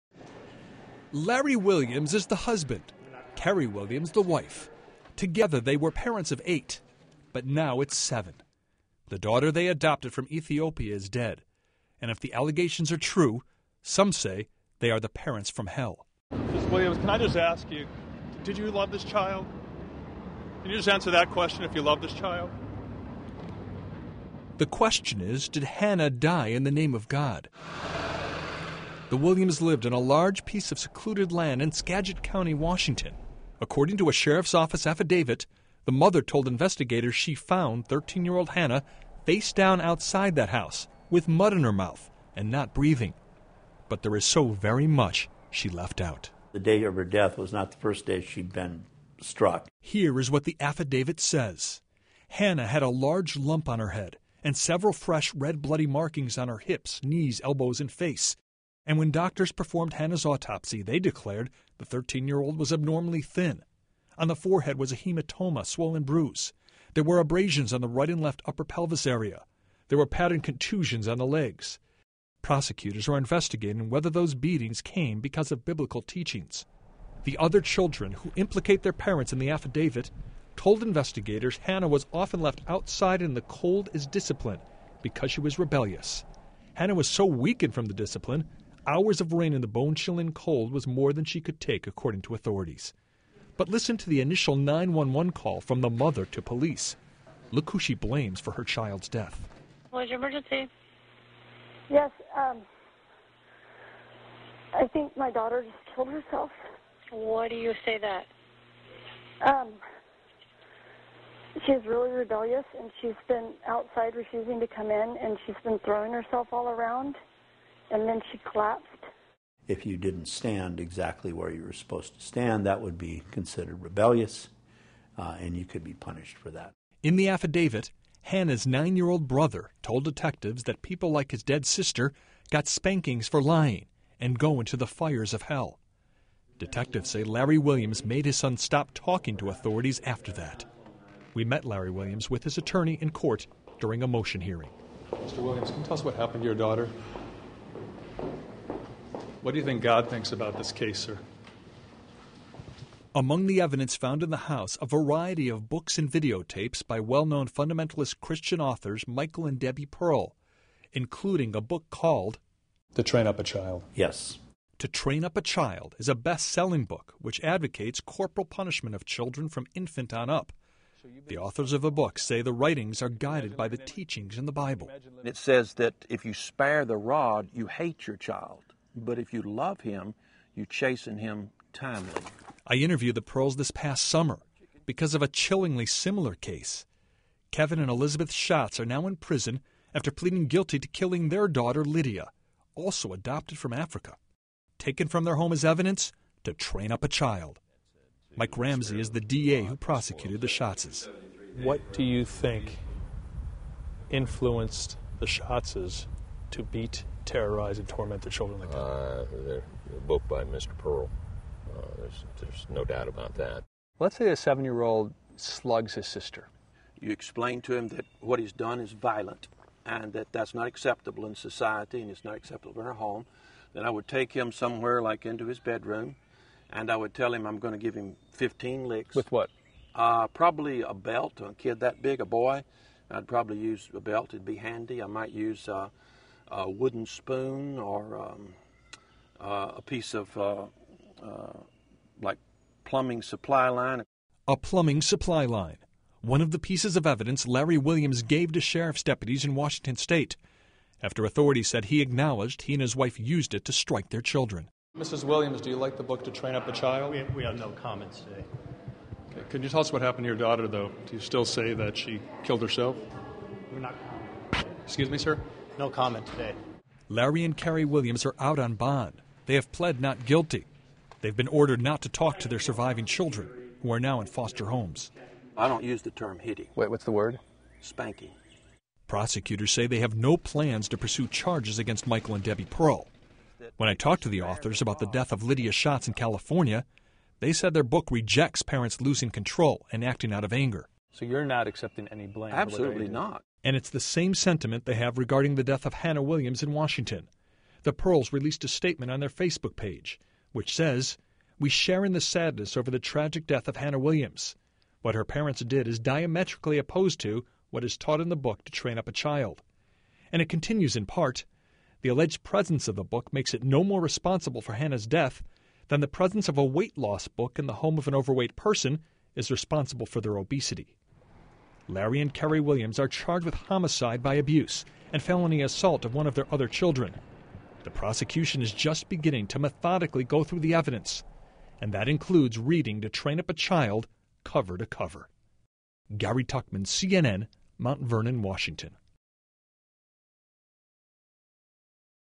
Homicide in the name of God?
CNN's Gary Tuchman reports on a little girl who's now dead and the parent who disciplined her.